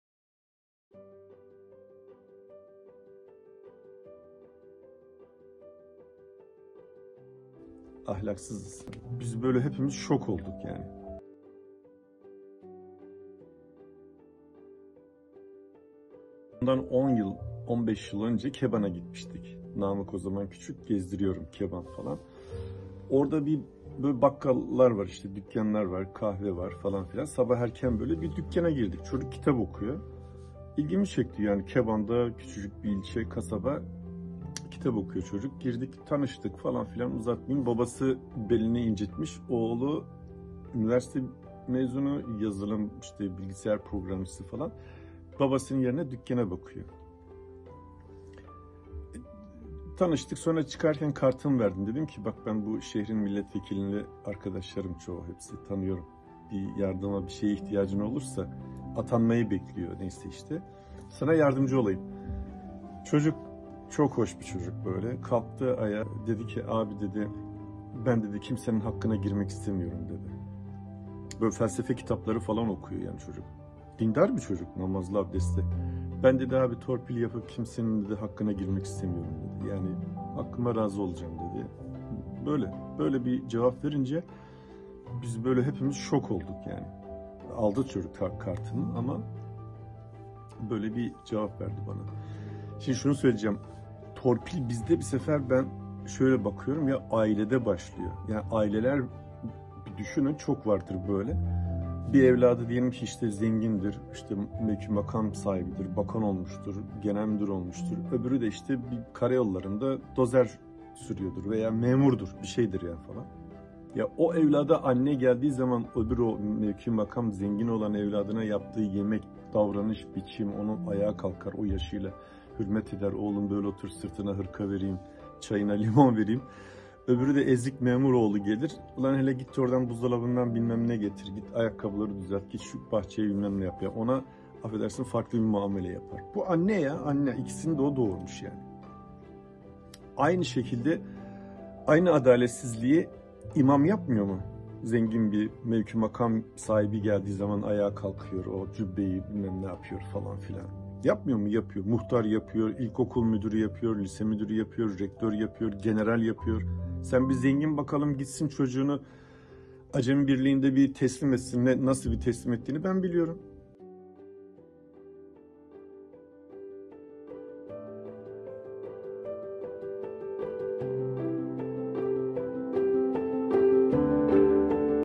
Torpil Ahlakı!
Şöyle der Tolstoy: 'İnsanoğlunun değeri bir kesirle ifade edilecek olursa; payı gerçek kişiliğini gösterir, paydası da kendisini ne zannettiğini. Payda büyüdükçe kesrin değeri küçülür.' İşte ahlak da paya yapılan doğru ve zamanında yükleme ile kıvam bulur ve kesrin değerini artırır.